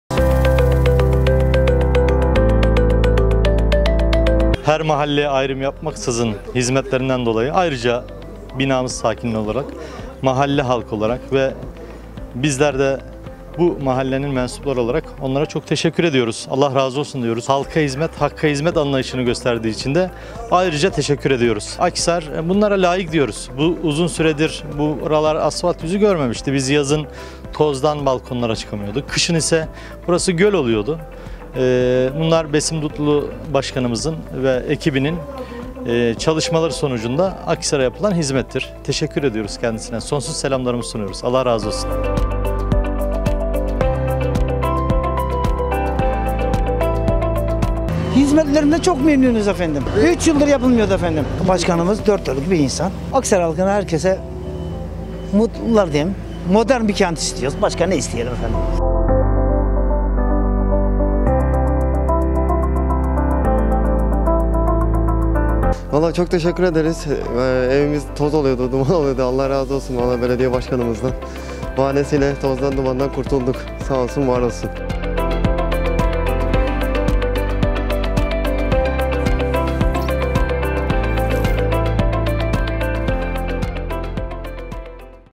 Hep birlikte güzel şehrimizi daha yaşanabilir bir yer yapacağız. 😊 | Akhisar Belediyesi
Daha modern ve daha güzel bir Akhisar için kent genelinde çalışmalarımız tüm hızıyla devam ediyor. Halkımızın memnuniyeti ve mutluluğu bize her zaman güç ve çalışma azmi veriyor. Hep birlikte güzel şehrimizi daha yaşanabilir bir yer yapacağız. 😊